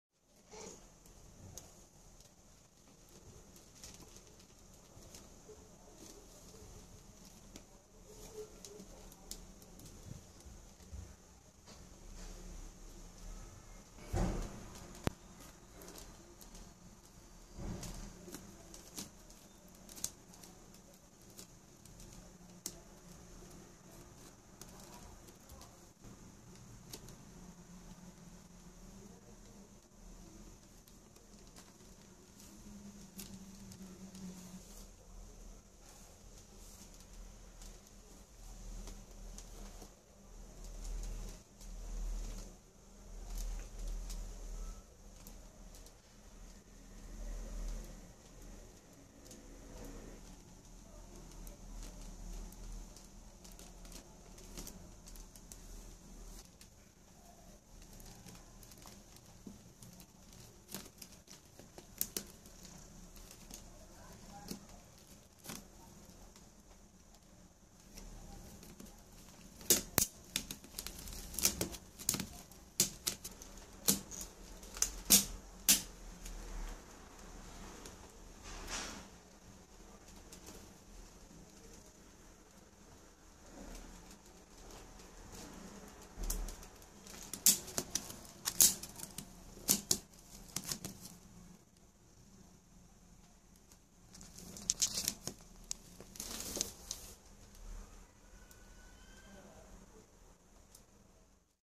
The Basket XXIV
Rising from an armadillo sleeping position Schrödinger enjoys being petted. Today he especially likes to be "tickled". As soon he has enough, he slips back in the armadillo state and dozes off.